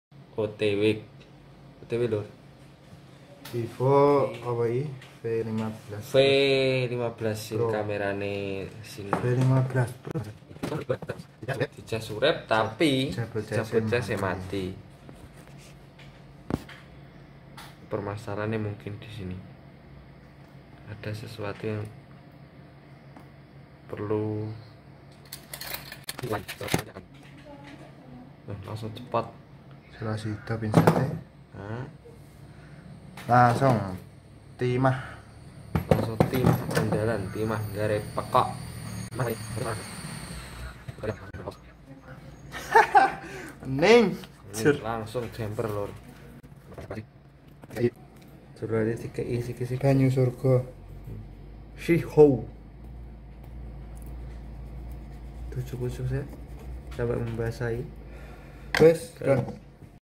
Vivo V15 Pro Restart (Battery Fuse Change)
- Vivo v15 pro restart
- Normal with charger
- Jumper batt fuse (replace recommended)
- Fun
- Done